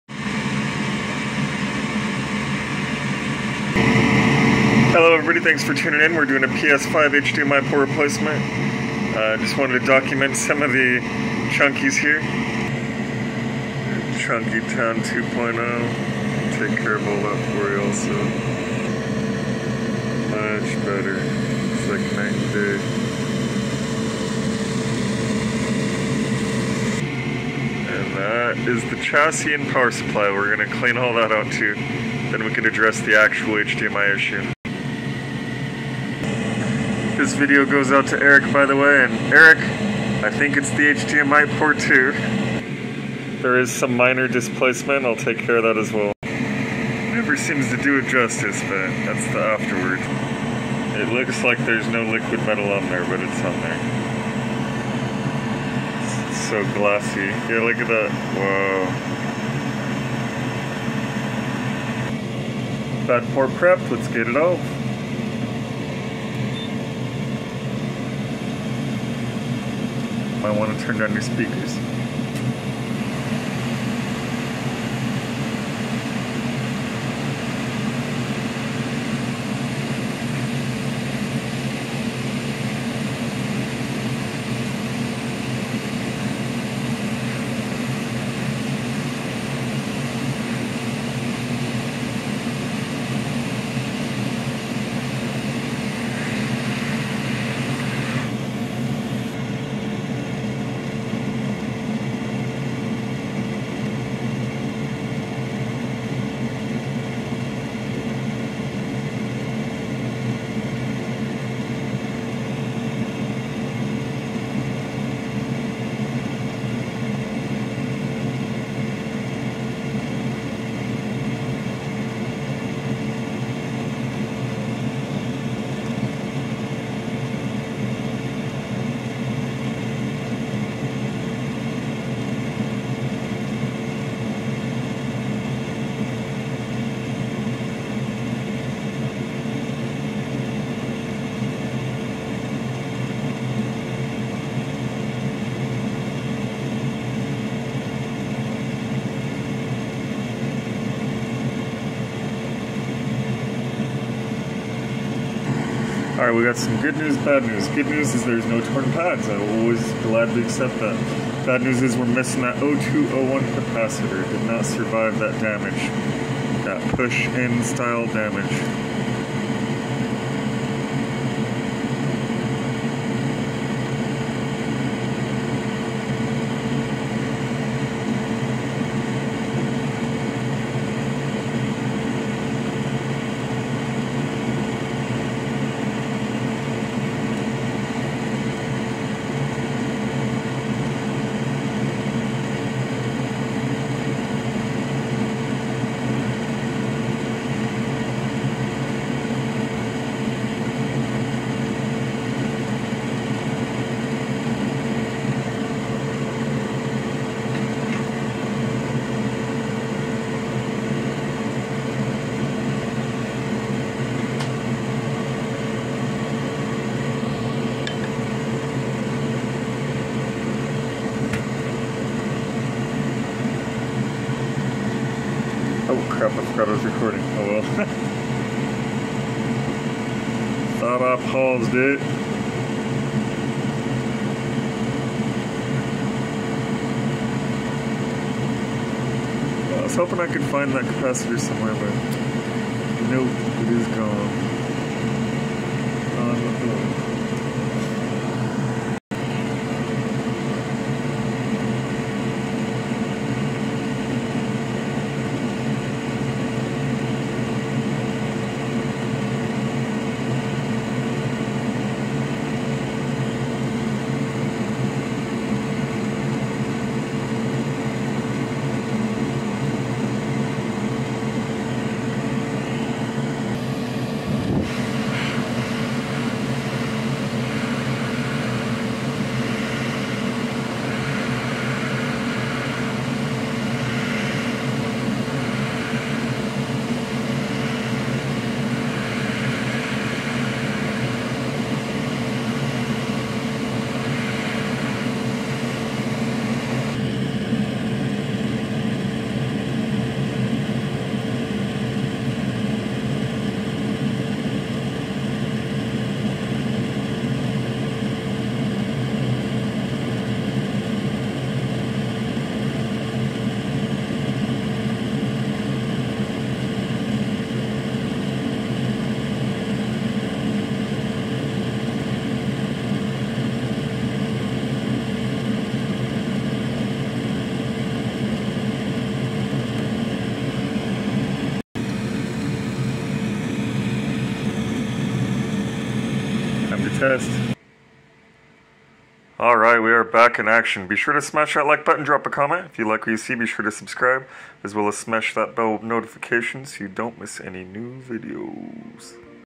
PS5 No Video Repair - (0965)
Client brought in a PS5 with no Video. He thinks it's the HDMI Port. Is he right? Is it something else? Can we fix it!? Watch and see!

CA Electronics Repair License #93621

SBL Games, Inc. 🎮
(2nd Floor, Suite 206H)

Business Hours:
Monday - Saturday
12 PM - 7 PM (PST)